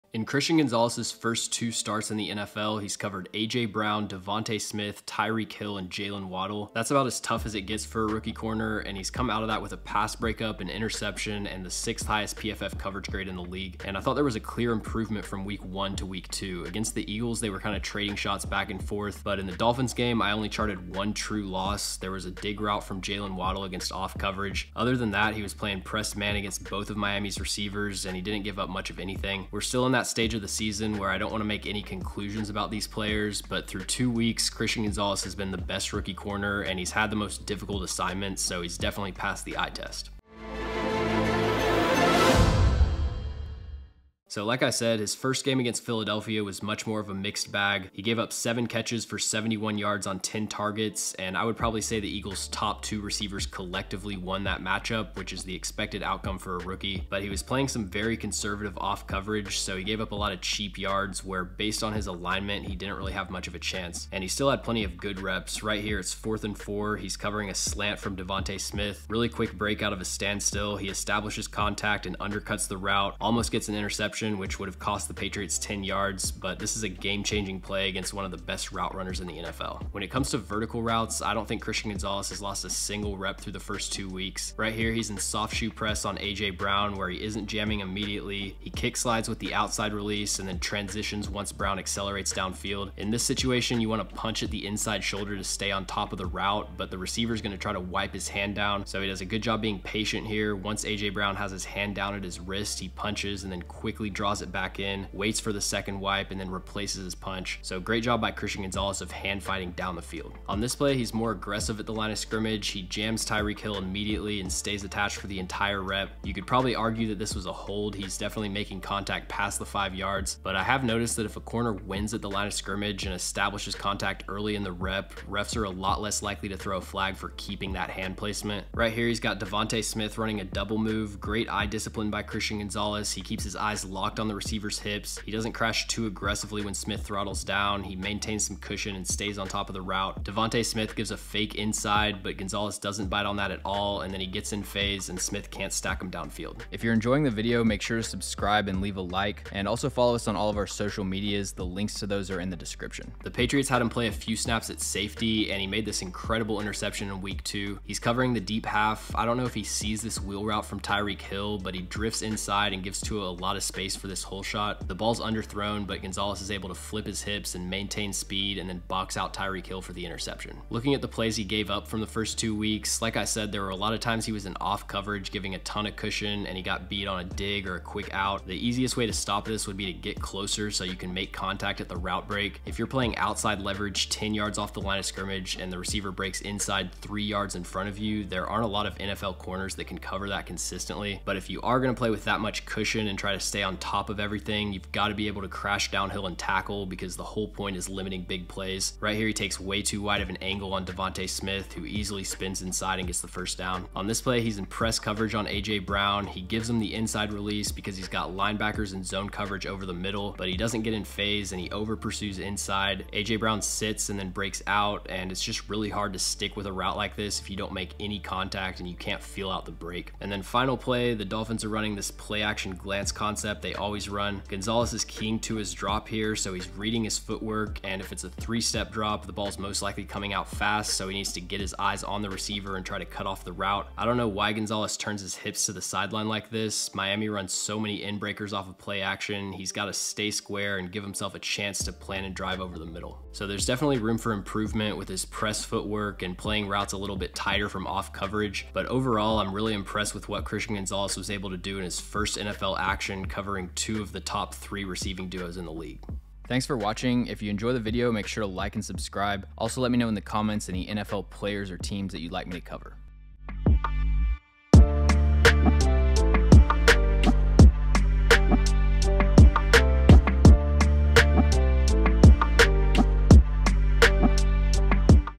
Patriots CB Christian Gonzalez is Passing the Eye Test: Film Breakdown
In this film breakdown, we take a look at New England Patriots rookie CB Christian Gonzalez, who has been solid through his first 2 games, despite having to cover AJ Brown, DeVonta Smith, Tyreek Hill, and Jaylen Waddle. He has 1 INT, 1 PBU, and the 6th highest coverage grade in the league. He has given up a lot of cheap yards underneath, however his coverage has actually been pretty strong downfield. He has plenty to improve on, but through 2 weeks, Gonzalez is passing the eye test.

*All film used is courtesy of NFL+ and the All-22 broadcasts associated with that service and subscription. No copyright is intended.*